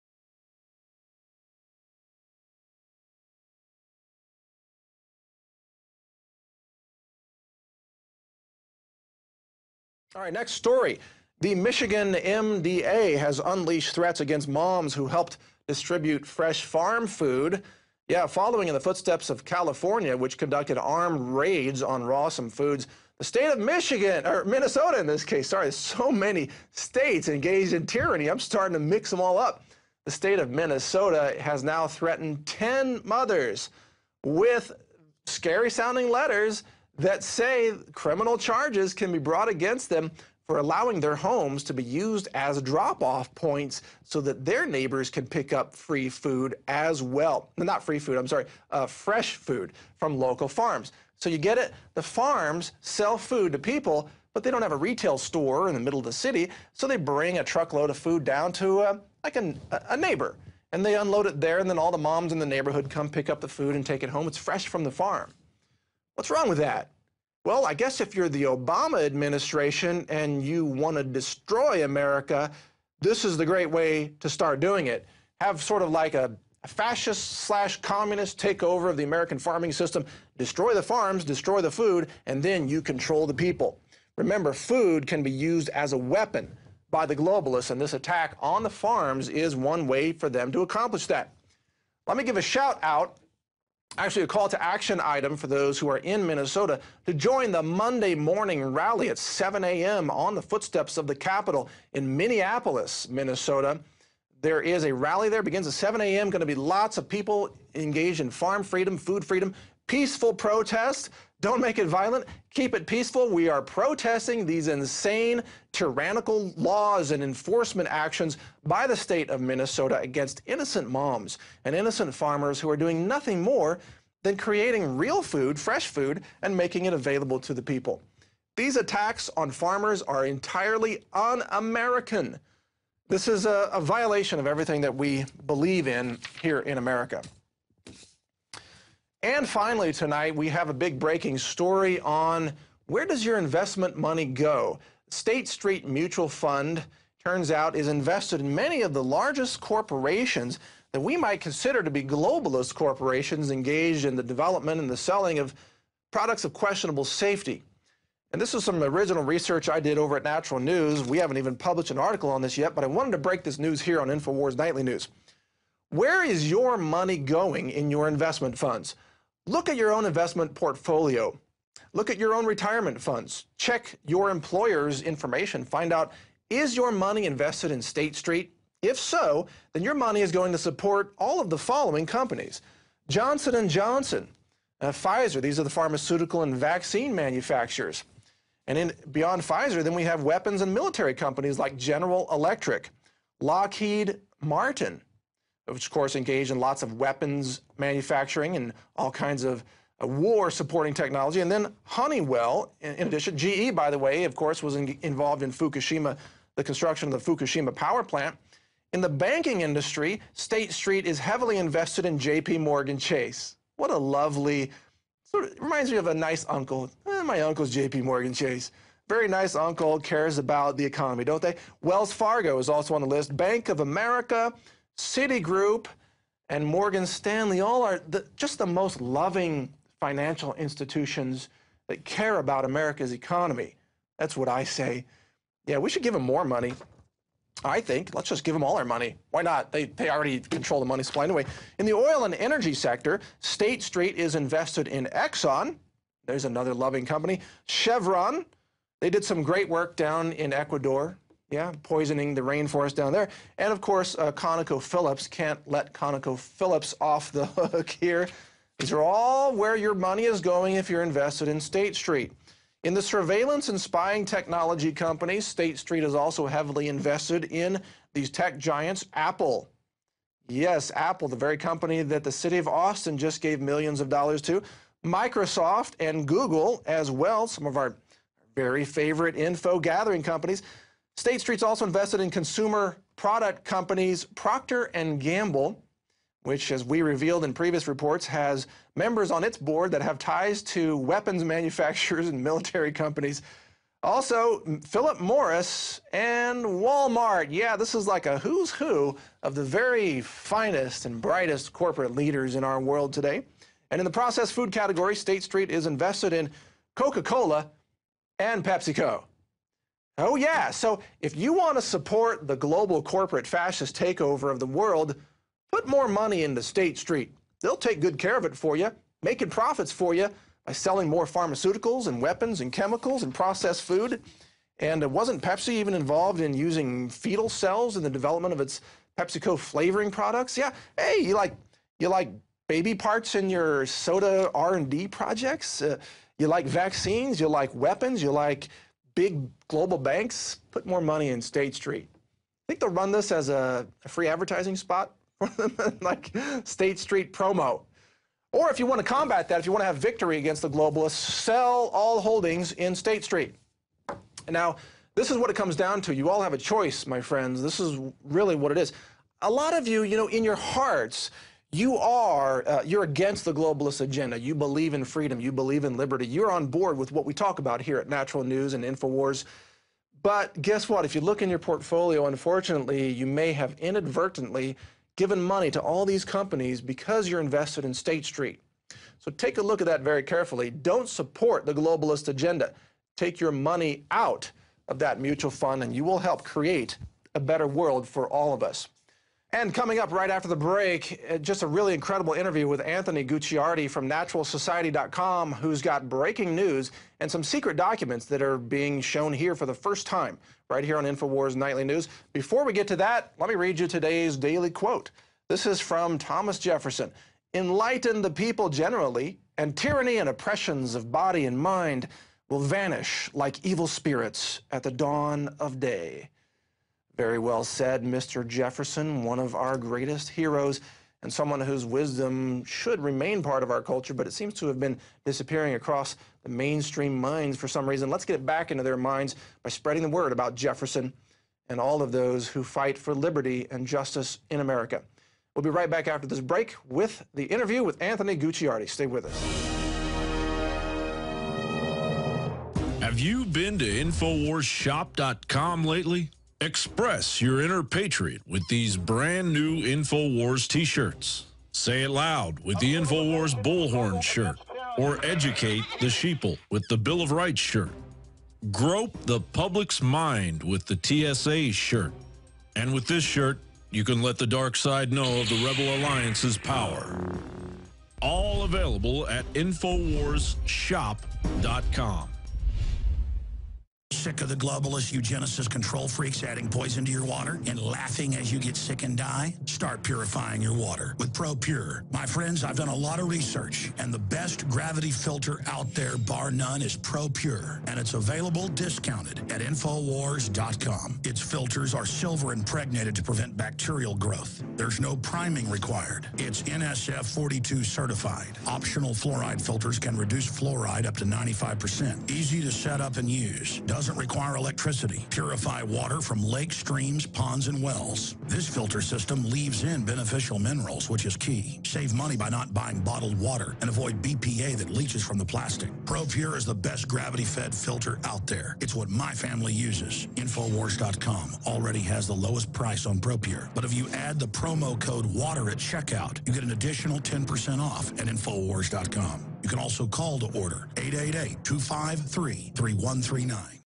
Health Ranger reports on MDA as it Unleashes Threats Against Raw Milk Moms
Health Ranger reports on MDA as it Unleashes Threats Against Raw Milk Moms. Great way to destroy the Farming systems, the food and eventually control people.   Stock up on food storage with non GMO, 25 year food storage with Go Foods Global